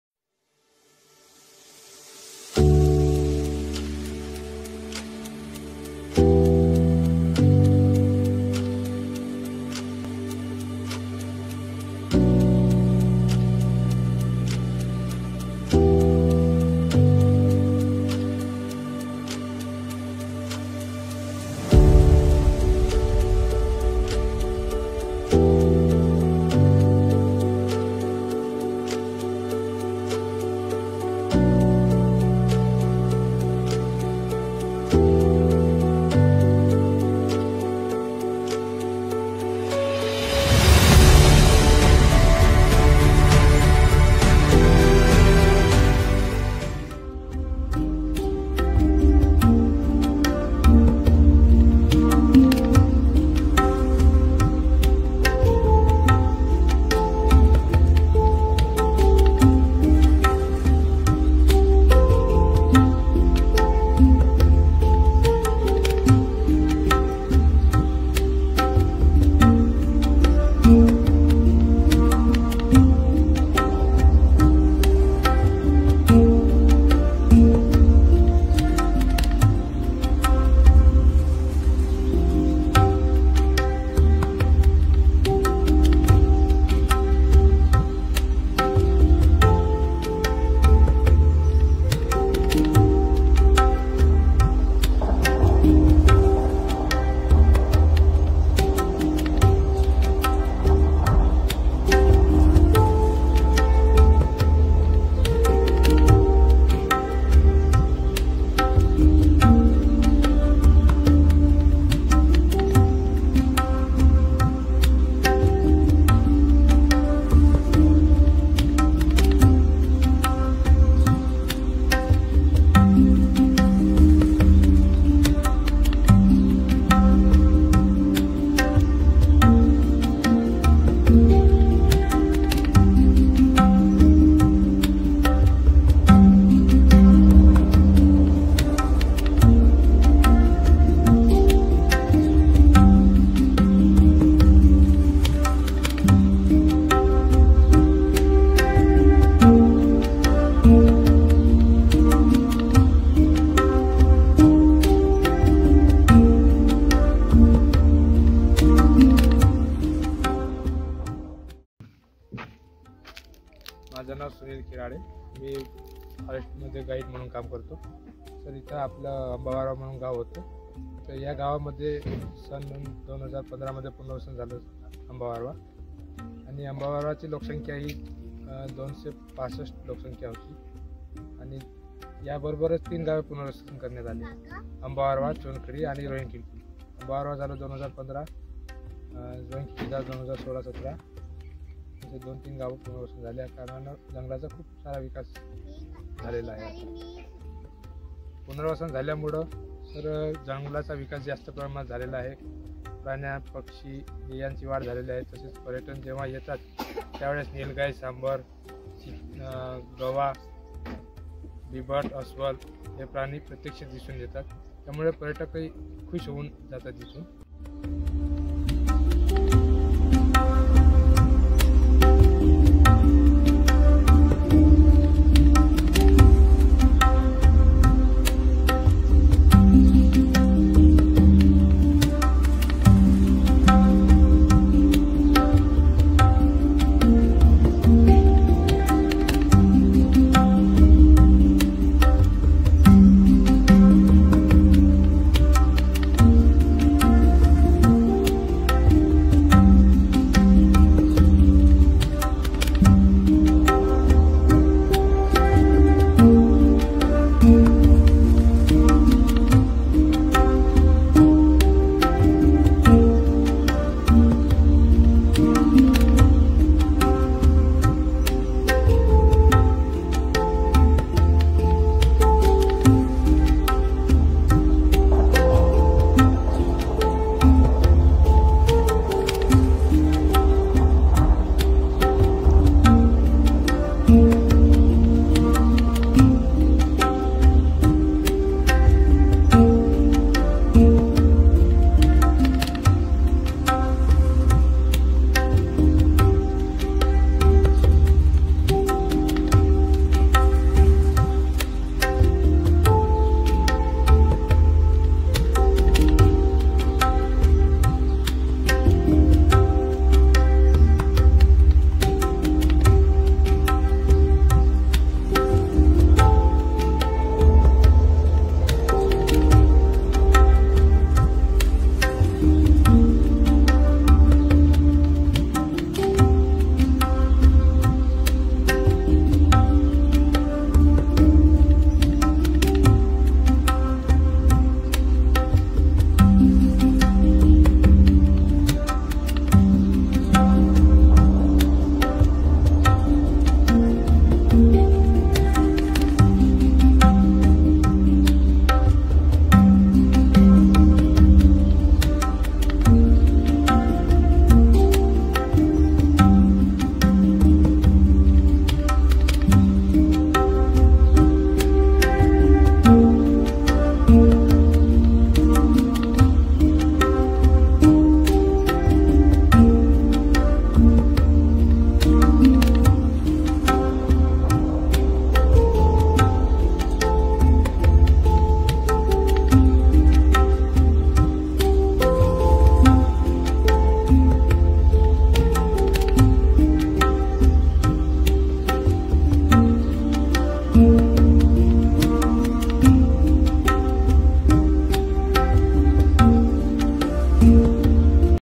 Ambabarwa Wildlife Safari | Melghat Safari | Jungle Safari
An incredible safari expirience at Ambabarwa wildlife Sanctury !!!